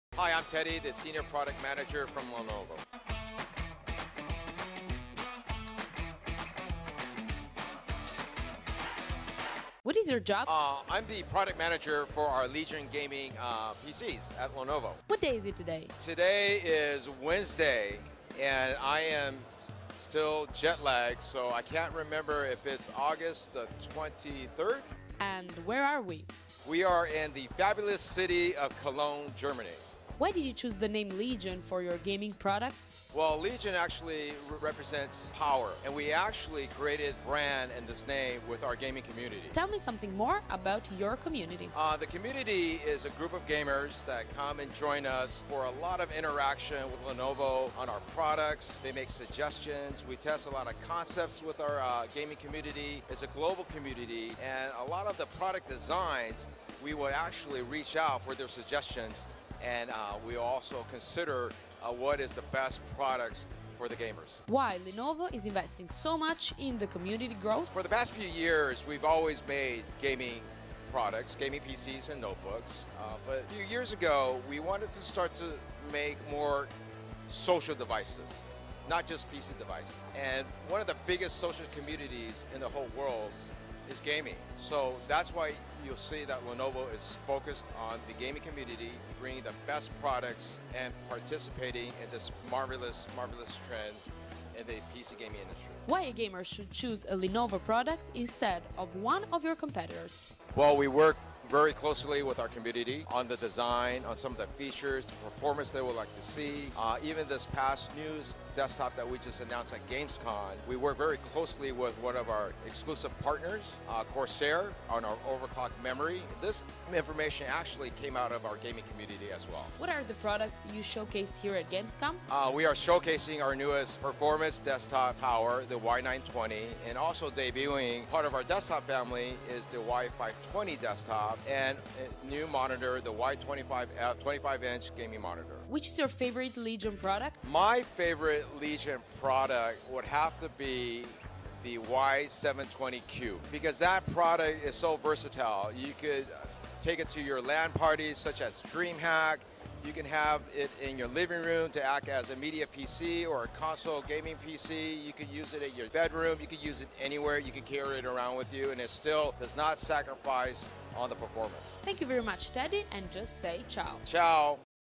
Legion: perché i PC da gaming di Lenovo si chiamano così? [ENG sub iTA]
Ci siamo sempre chieste come mai i PC da gaming di Lenovo si chiamino proprio "Legion". Per scoprirlo, abbiamo intervistato Teddy Lee di Lenovo.